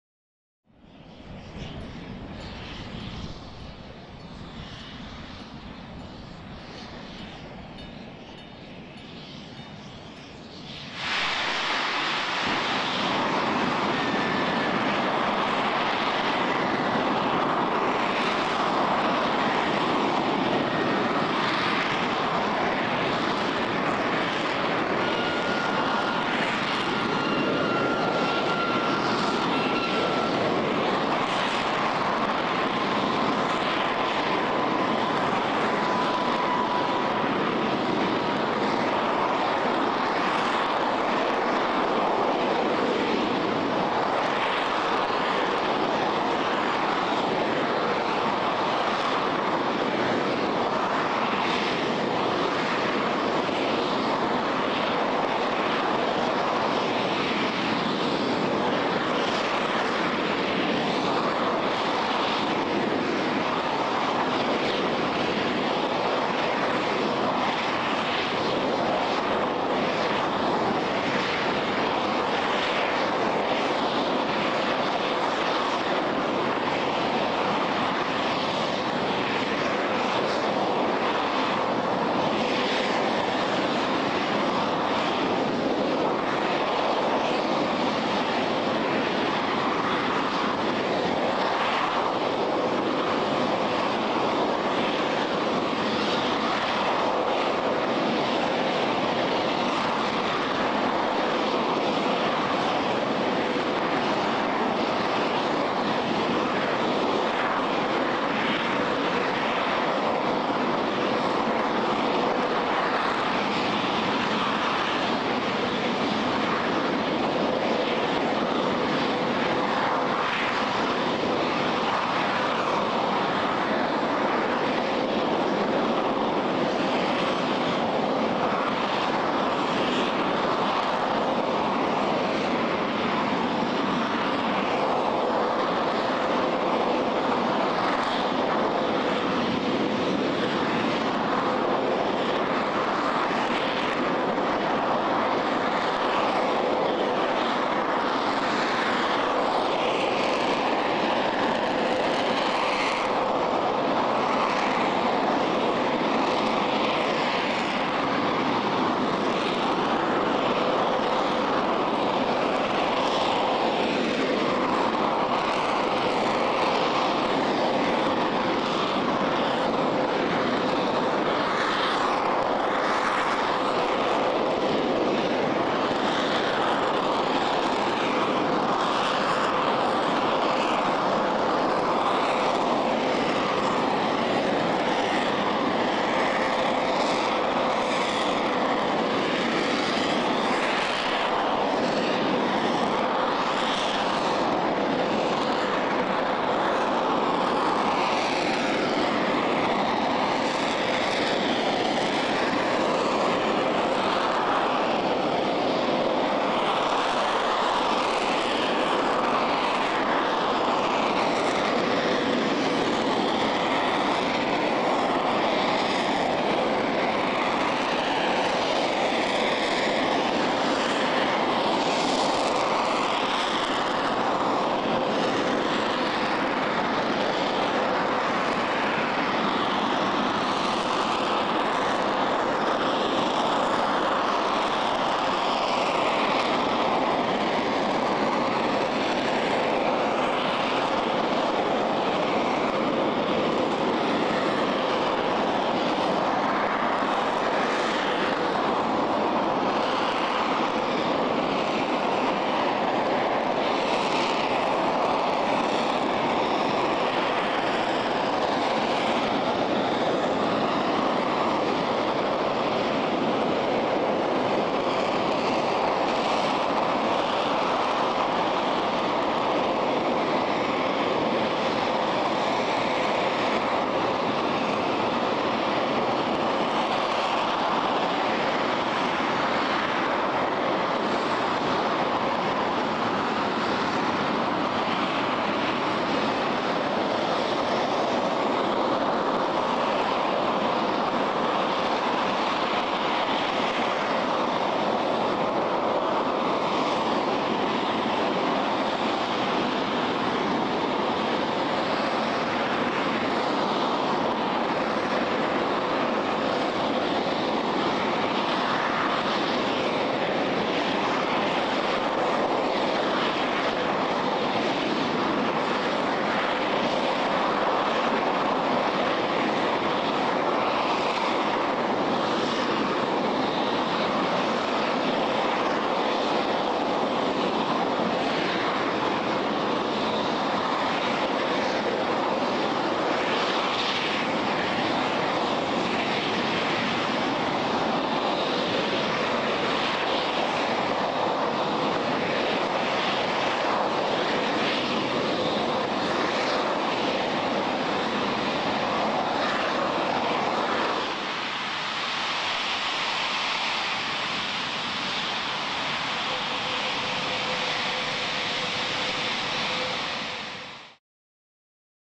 NASA Continues J-2X Powerpack Testing
NASA conducted a long duration test of the J-2X powerpack, 340 seconds total, at the Stennis Space Center in southern Mississippi on May 10, marking another step in SLS development, the next-generation rocket that will carry humans deeper into space than ever before.